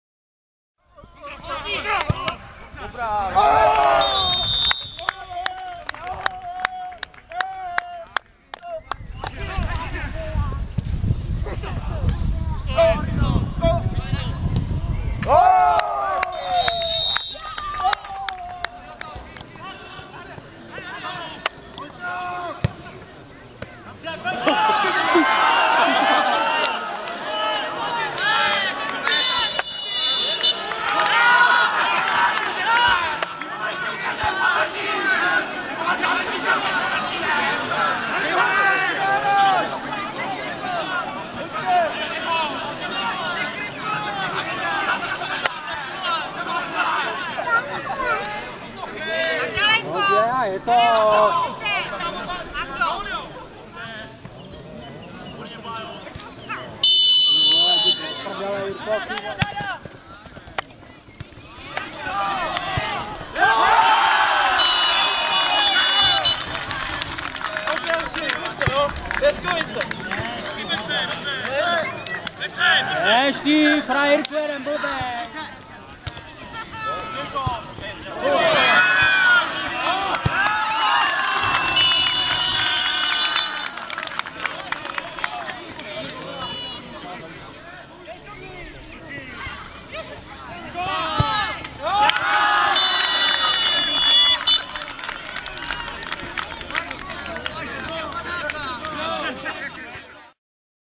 7.7.2009 - Memoriál Mirka Štolce 2009 - Jeviněves [HQ]
Utkání AFK Eletis Lužec - Sokol Nová Ves a Slovan Horní Beřkovice - Sokol Jeviněves